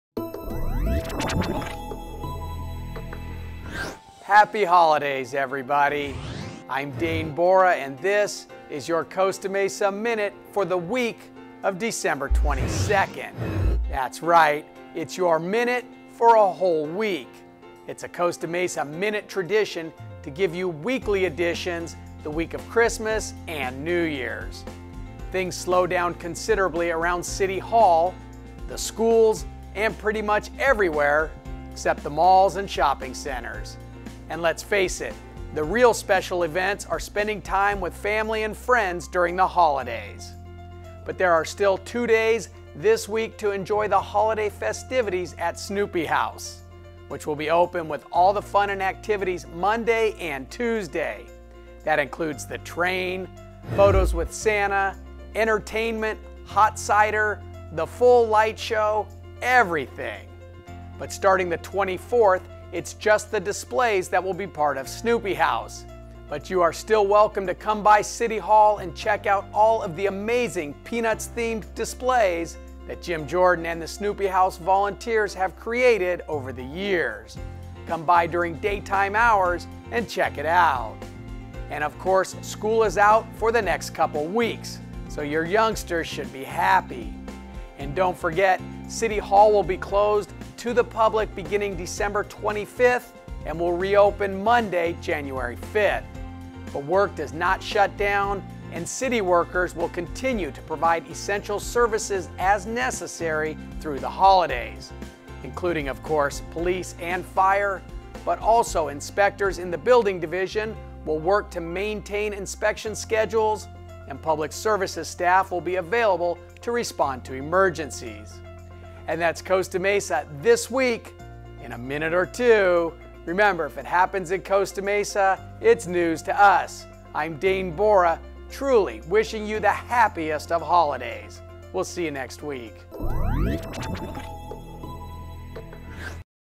Costa Mesa Minute Dec 22
Just a couple days left to enjoy the festivities at Snoopy House, City Hall prepares for the holidays, and more on today's Costa Mesa Minute.